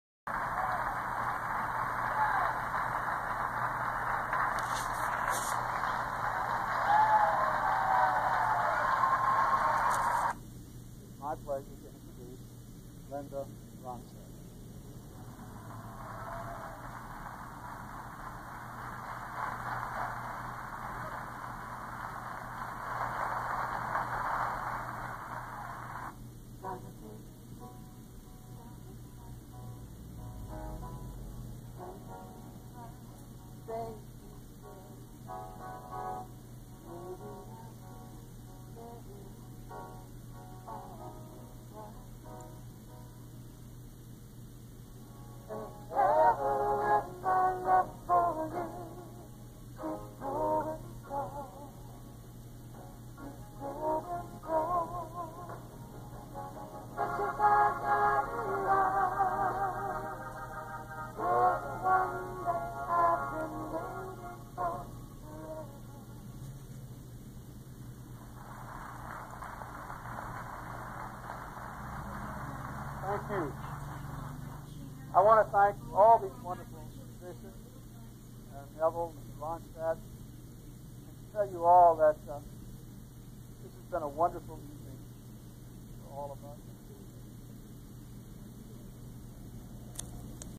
RONSTADT white house visit
Short vid of Linda meeting Hillary & Bill Clinton at the white house